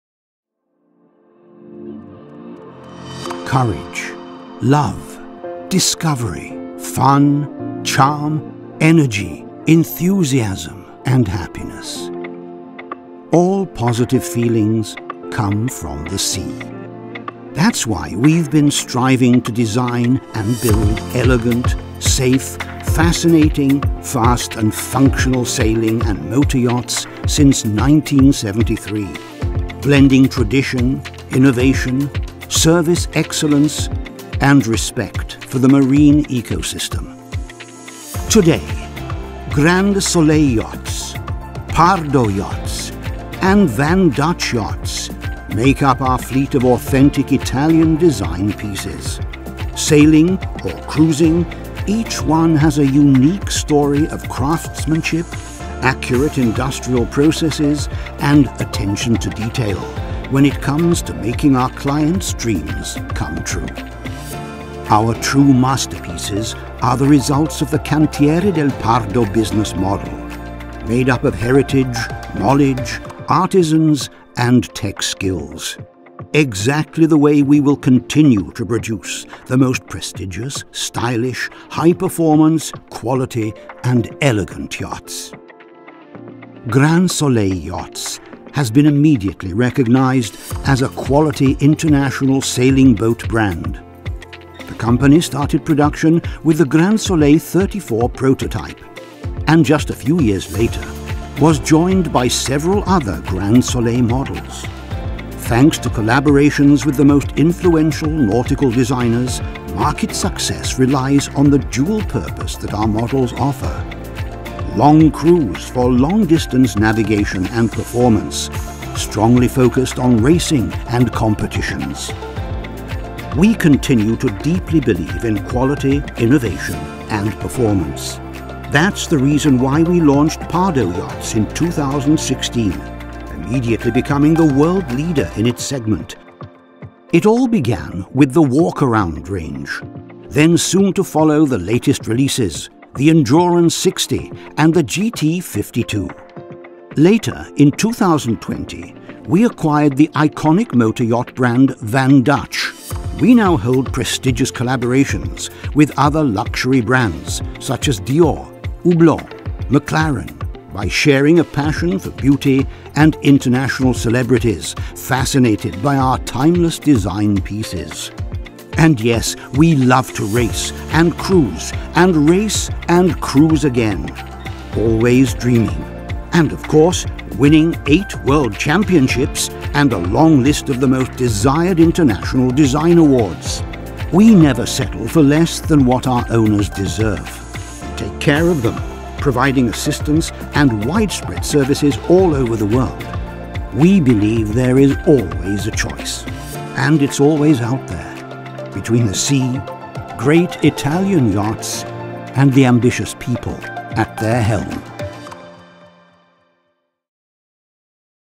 Pardo Yachts - Cantiere del Pardo 2022
Découvrez Cantiere del Pardo avec cette vidéo de présentation du chantier de PARDO Yachts.
Entrez en immersion dans l'univers de PARDO Yachts et tout le savoir faire italien.
À travers cette vidéo, laissez-vous porter par le service d'excellence de ces bateaux, de leur innovation, du respect de l'environnement marin et de la tradition italienne maîtrisée depuis 1973.
Discover Cantiere del Pardo with this video presentation of the PARDO Yachts shipyard.
Immerse yourself in the world of PARDO Yachts and all the Italian know-how.
Through this video, let yourself be carried away by the excellent service of these boats, their innovation, respect for the marine environment and the Italian tradition mastered since 1973.